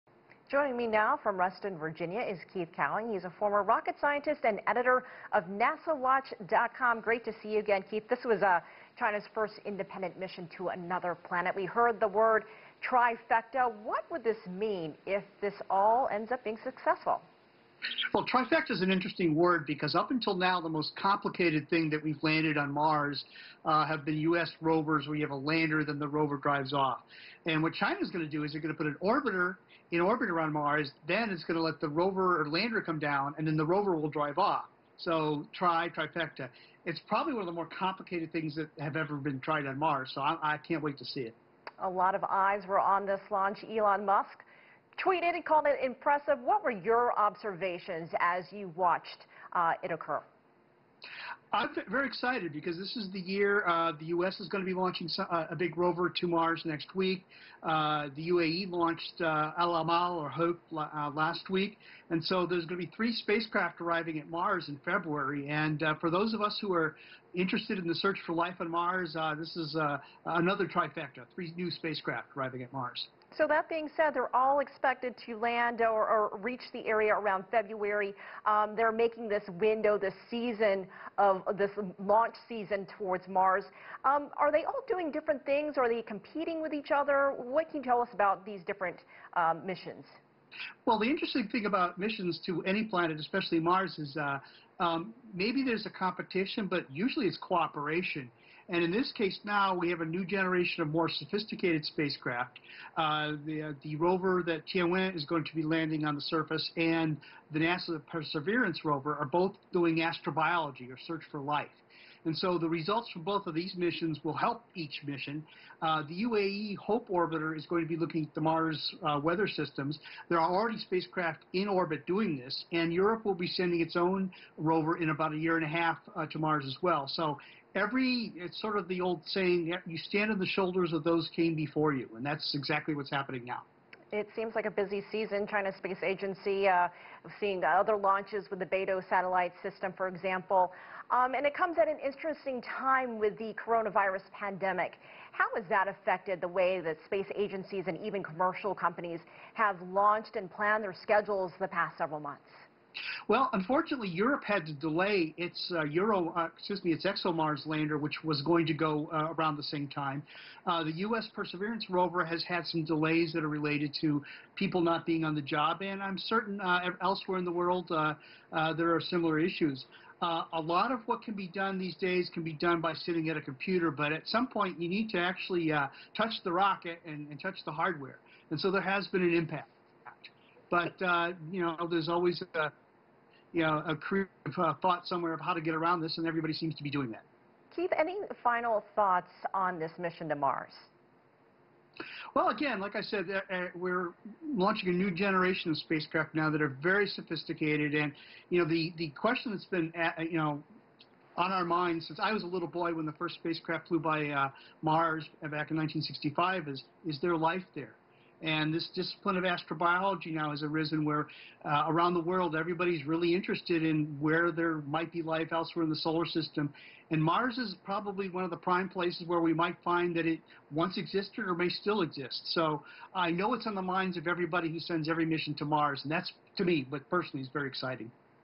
Former rocket scientist on China's first sole mission to Mars
Former rocket scientist Keith Cowing talked about China's successful launch in its first independent mission to Mars.